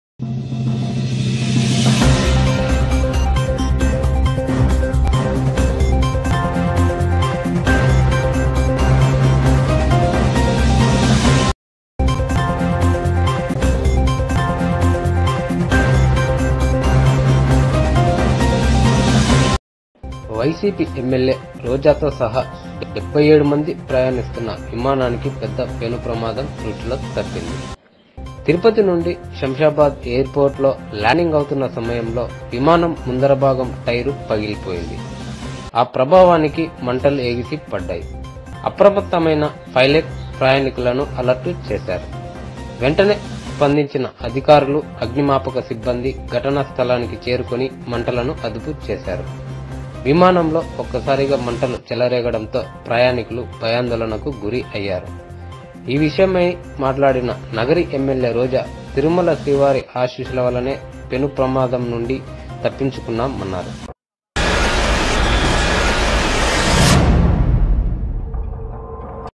YSRCP MLA Roja Escapes Major Flight Accident - Shamshabad Air port - MLA Roja || KNEWS TELUGU
BREAKING NEWS
POLITICAL NEWS
FUNNY NEWS
STUDY PROGRAMES
HEALTH TIPS
DISTRICT NEWS
LADES SPECIAL PROGRAMES

 MORE UP DATES WATCH KNEWS TELUGU CHANNEL